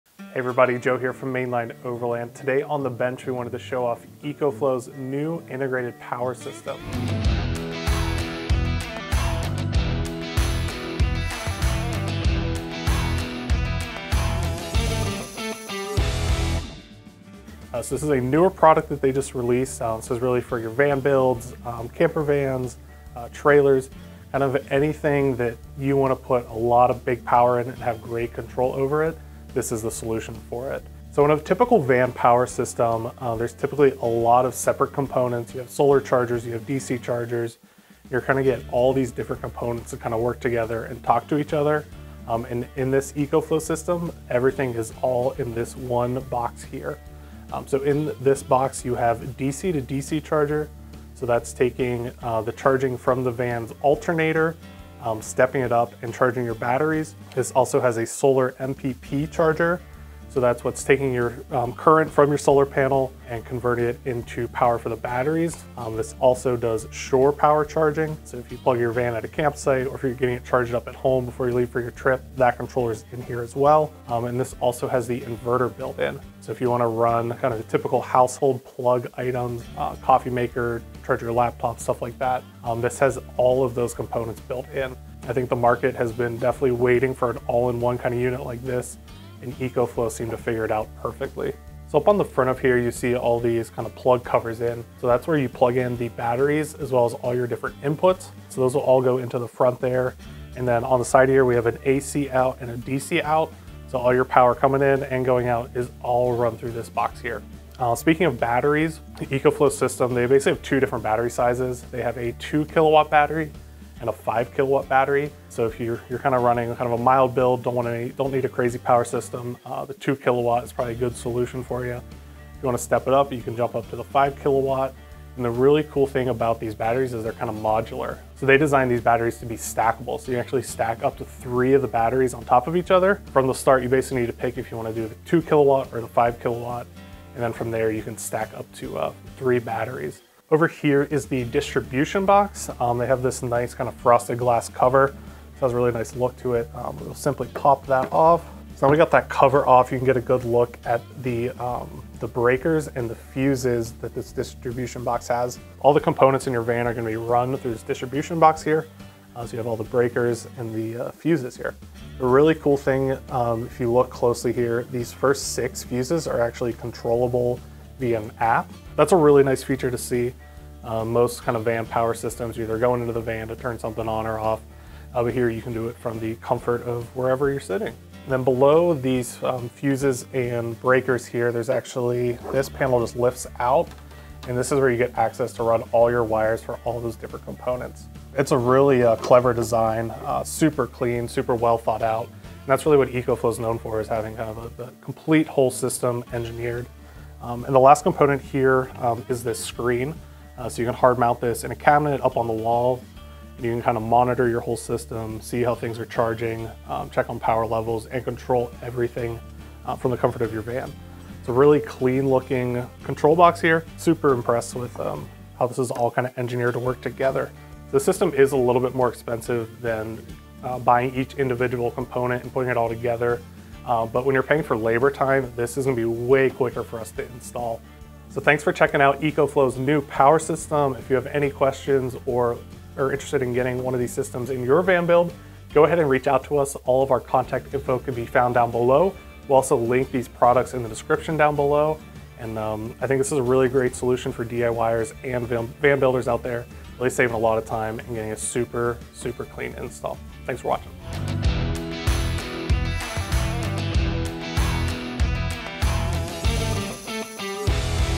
ECOFLOW COMPLETE VAN POWER SOLUTION
EcoFlow Power Kits are the world's first compact modular power solutions for tiny homes, camper vans, and RVs. With a plug-and-play design, customizing your energy is quicker and easier than ever. Extremely easy to install and saves a ton of space by combining multiple systems in one. Everything is modular meaning you can add to the system if your power requirements change.